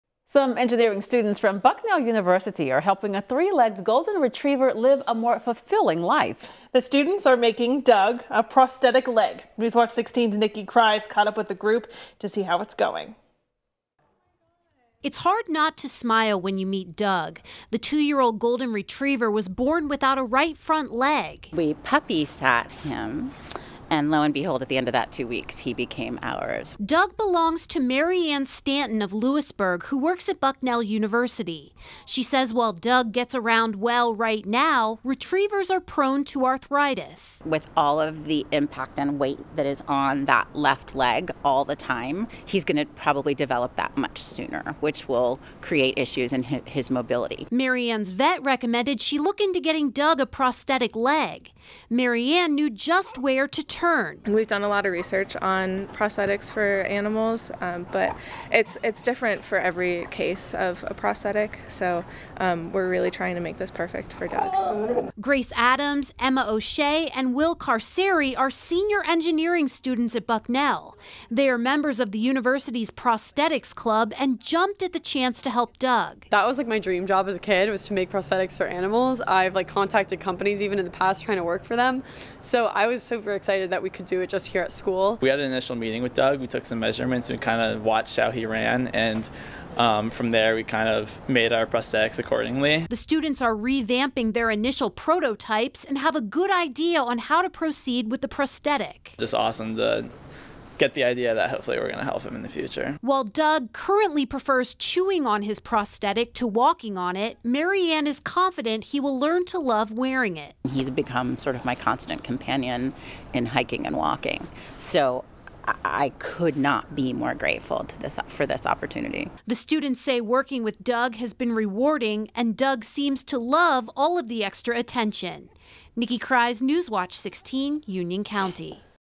Retriever rehab: Students build leg for Doug the golden retriever
Some engineering students from Bucknell University are helping a three-legged golden retriever live a more fulfilling life.